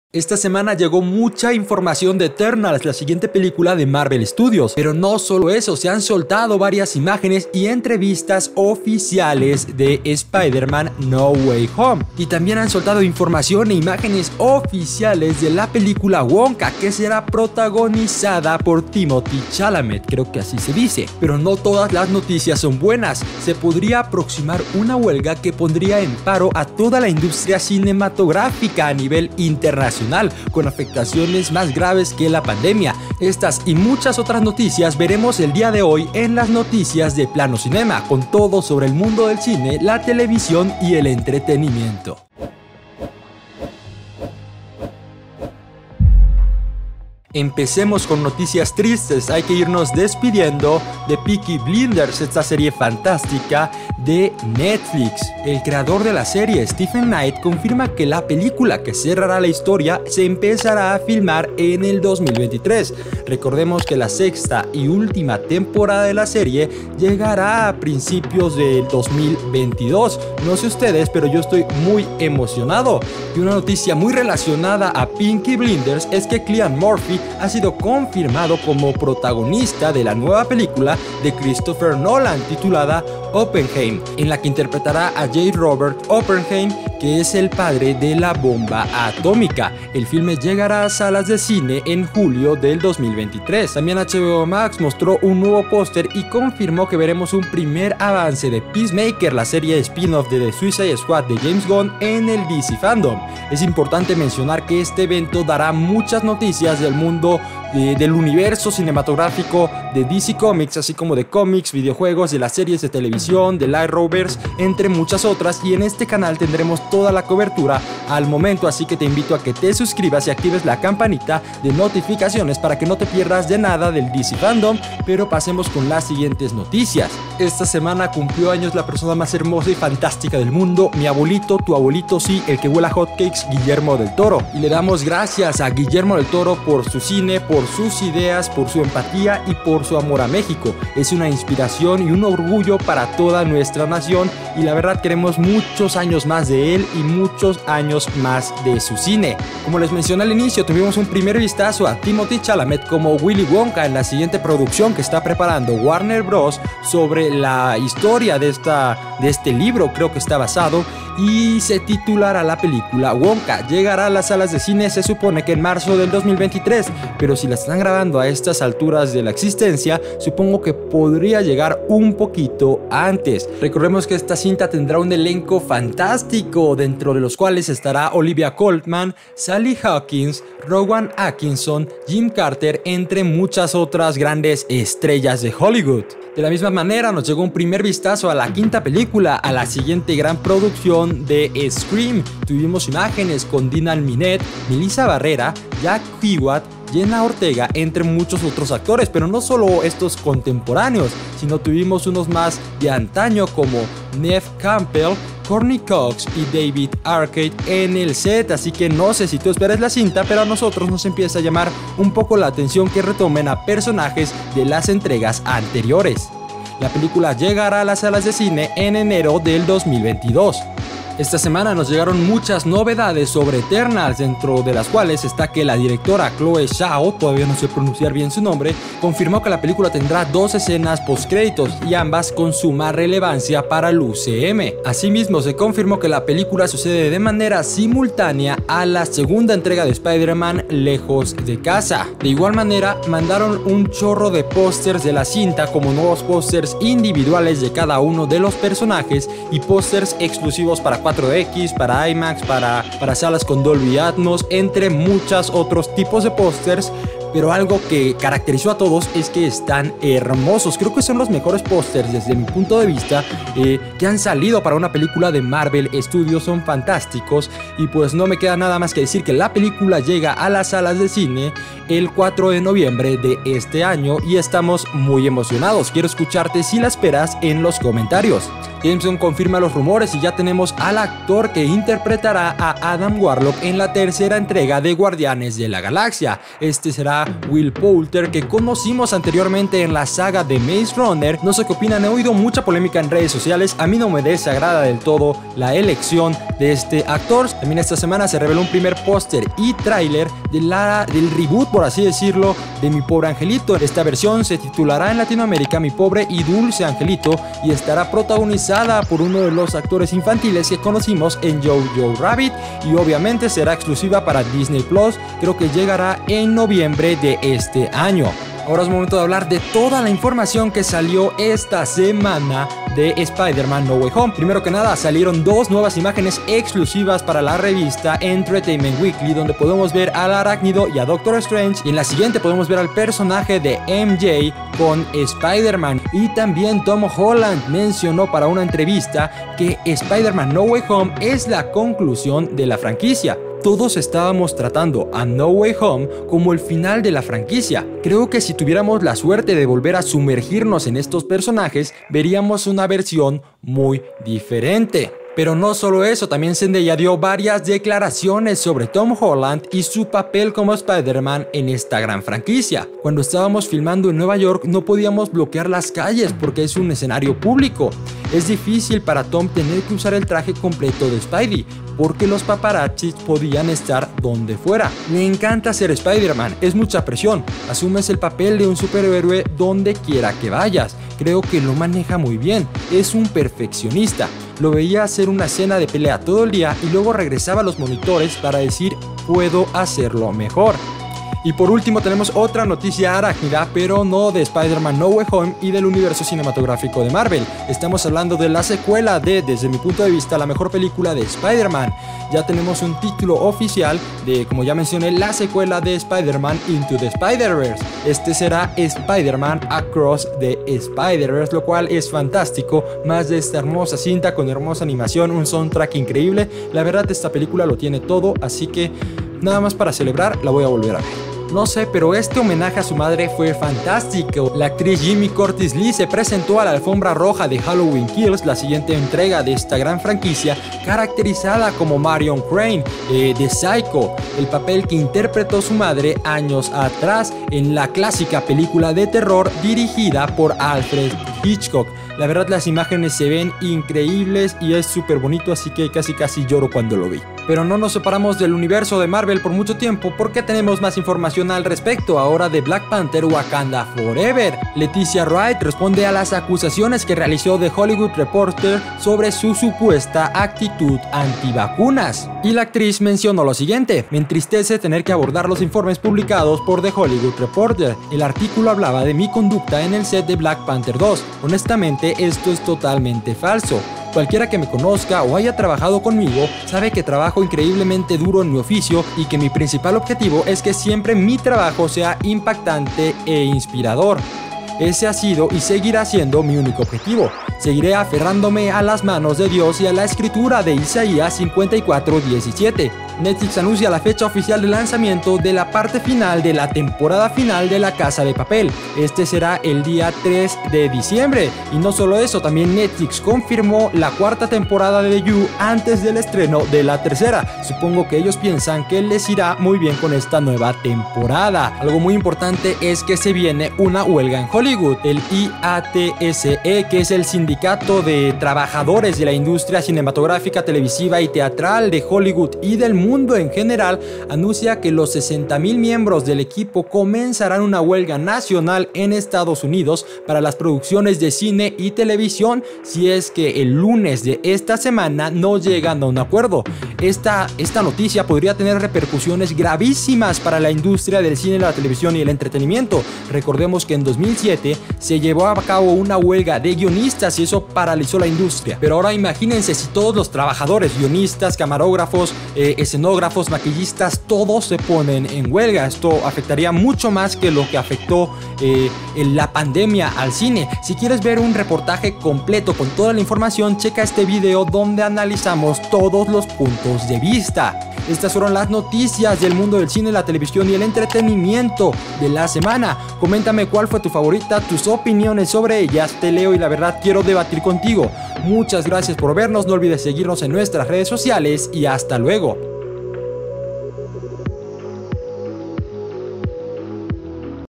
MÁS de NO WAY HOME y ETERNALS 🕷⭐/ ¡HUELGA EN HOLLYWOOD!🔥/ Timothée como WONKA🍫 | Noticias de cine 🎬
¡Adiós PeakyBlinders! ¡TimothéeChalamet será WillyWonka! ¡LetitiaWright y BlackPanther: WakandaFOREVER! 😱TODO esto y MÁS en las noticias de Plano Cinema. 🔥🎬

SI llegaste AQUÍ significa que perteneces al 1% de las personas que lee las descripciones HASTA EL FINAL comenta ¨ ¨ 🚨